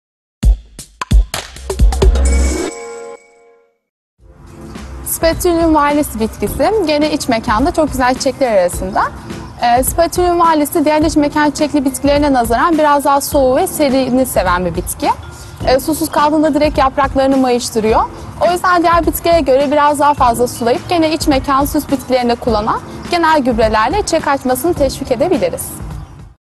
Barış çiçeğinin (Spathiphyllum) bakımı nasıl yapılır?
Barış çiçeği suyu ve serin ortamları seven bir bitki.